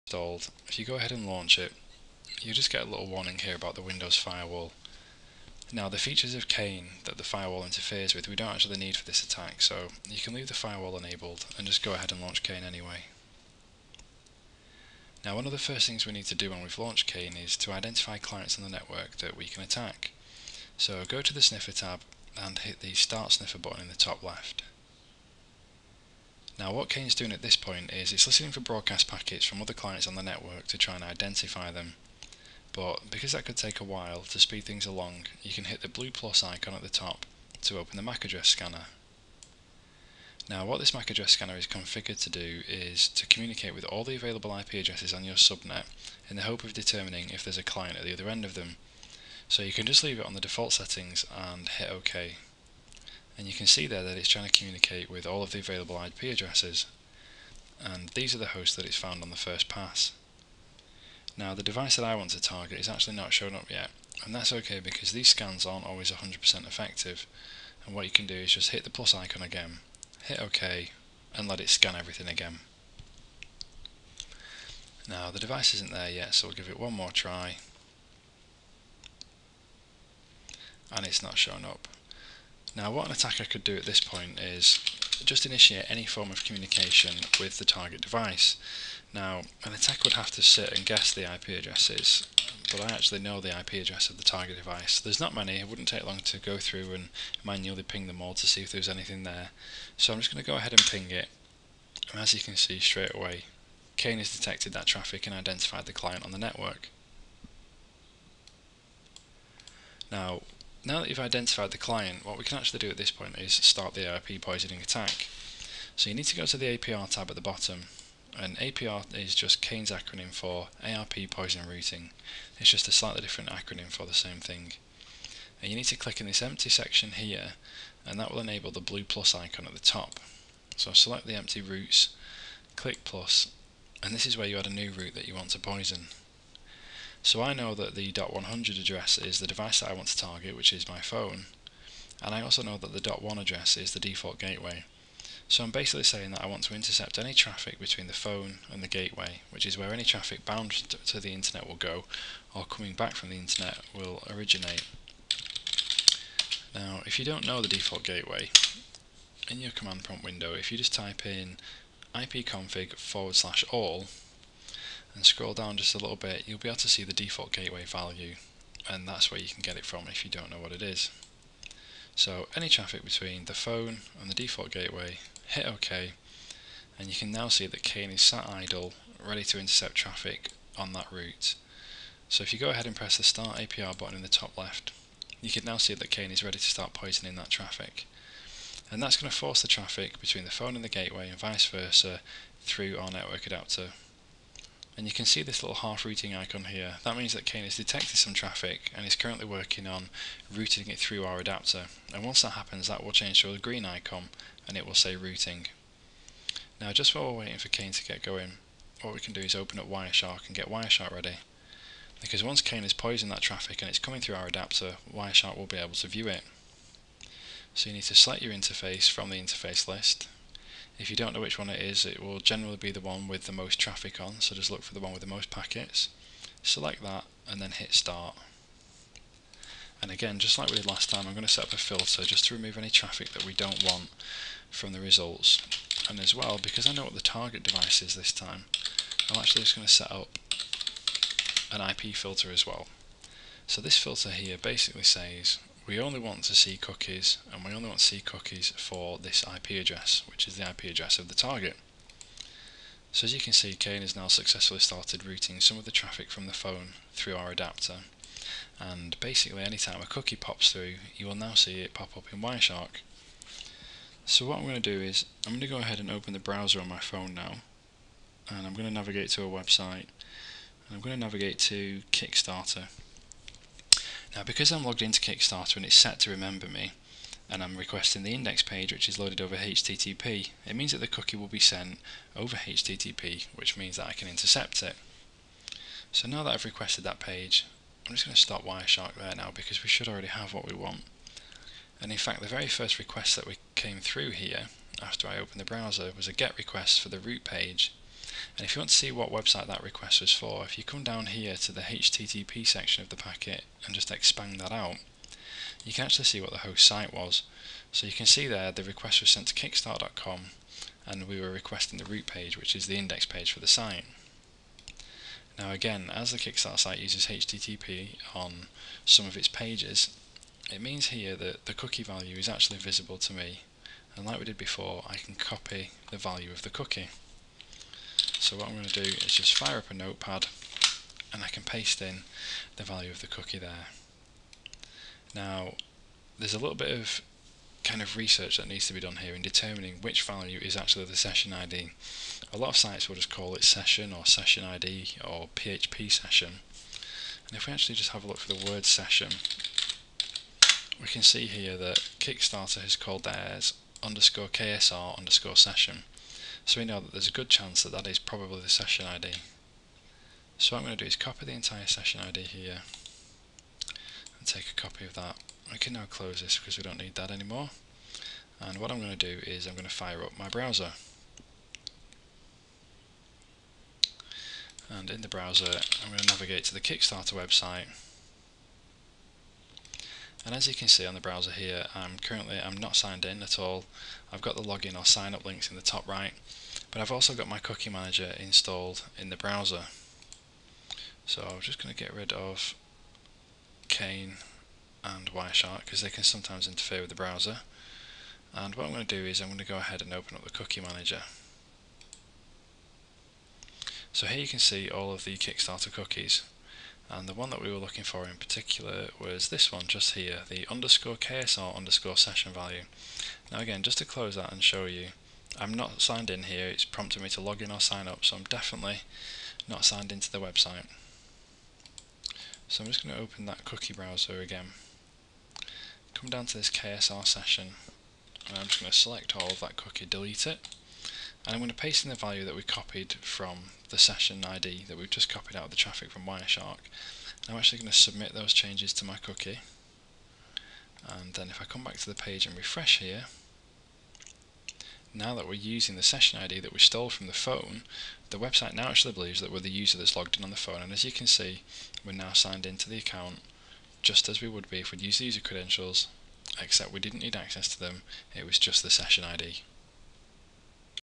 Session Hijacking With ARP Poisoning Using Cain and Abel
This Video describes to perform Session Hijacking With ARP Poisoning Using Cain and Abel in Windows.